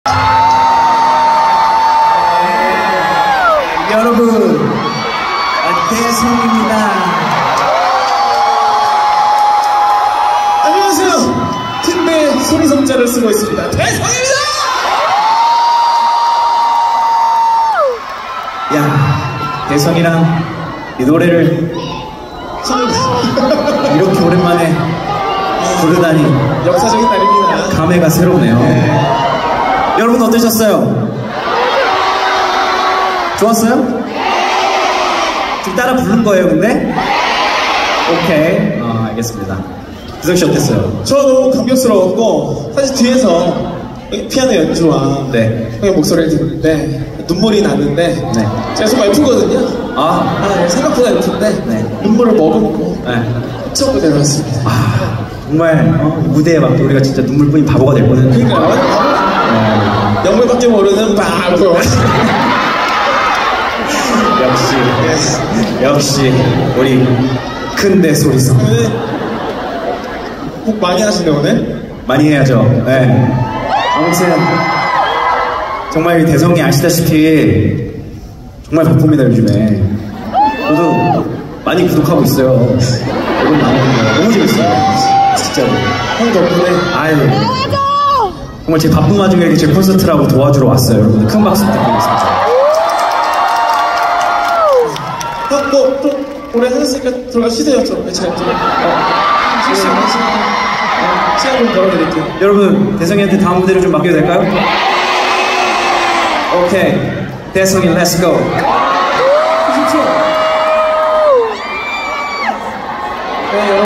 20240831 Taeyang Daesung brotherhood talk 태양 대성 이야기 at THE LIGHT YEAR Concert
4K Entertainment News Report